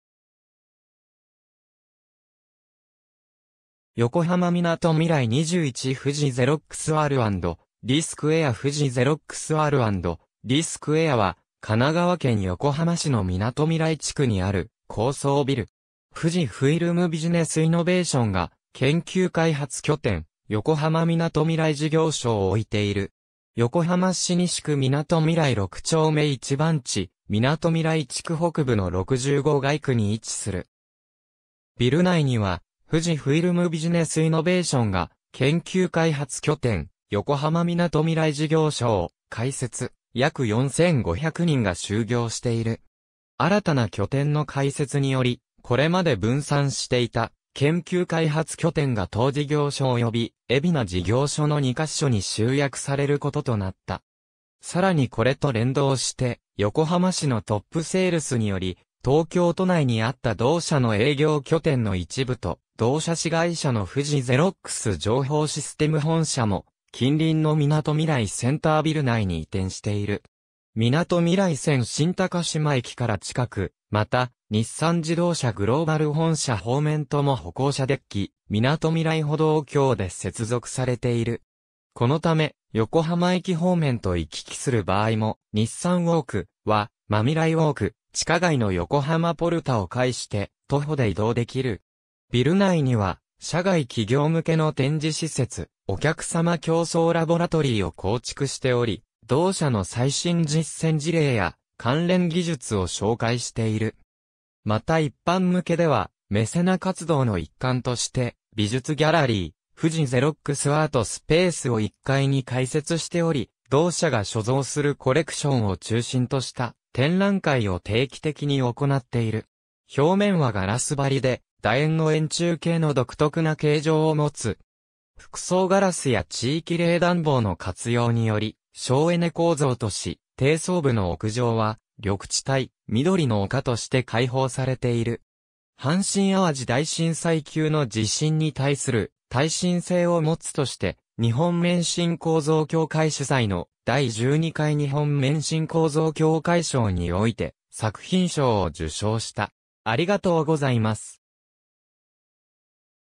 富士ゼロックスR&amp;Dスクエア
横浜みなとみらい21 」 富士ゼロックスR&Dスクエア 富士ゼロックスR&Dスクエア（ふじゼロックス アールアンドディ スクエア、英: FUJI XEROX R&D SQUARE）は、神奈川県横浜市のみなとみらい地区にある高層ビル。
富士フイルムビジネスイノベーション（旧・富士ゼロックス）が研究開発拠点「横浜みなとみらい事業所」を置いている。
横浜市西区みなとみらい六丁目1番地、みなとみらい地区北部の65街区に位置する。
ビル内には富士フイルムビジネスイノベーション（旧・富士ゼロックス）が研究開発拠点「横浜みなとみらい事業所」を開設（2010年4月より順次稼働）、約4,500人が就業している。
新たな拠点の開設により、これまで分散していた研究開発拠点が当事業所および海老名事業所の2箇所に集約されることとなった。
さらにこれと連動して横浜市のトップセールスにより、東京都内にあった同社の営業拠点の一部と同社子会社の富士ゼロックス情報システム本社（2020年4月に富士フイルムICTソリューションズと統合して現在は富士フイルムシステムズ）も近隣のみなとみらいセンタービル内に移転している。
みなとみらい線新高島駅から近く、また日産自動車グローバル本社方面とも歩行者デッキ「みなとみらい歩道橋」で接続されている。
このため、横浜駅方面と行き来する場合もNISSANウォーク・はまみらいウォーク、地下街の横浜ポルタを介して徒歩で移動できる。
ビル内には社外企業向けの展示施設「お客様共創ラボラトリー」を構築しており、同社の最新実践事例や関連技術を紹介している。
また一般向けでは、メセナ活動の一環として美術ギャラリー「Fuji Xerox Art Space」（富士ゼロックス・アートスペース/版画ギャラリー）を1階に開設（東京にて2003年より開設していたギャラリーを移設）しており、同社が所蔵するコレクションを中心とした展覧会を定期的に行っている。
表面はガラス張りで、楕円の円柱形の独特な形状を持つ。
複層ガラスや地域冷暖房の活用により省エネ構造とし、低層部の屋上は緑地帯「緑の丘」として開放されている。
阪神淡路大震災級の地震に対する耐震性を持つとして、日本免震構造協会主催の「第12回日本免震構造協会賞」において作品賞を受賞した。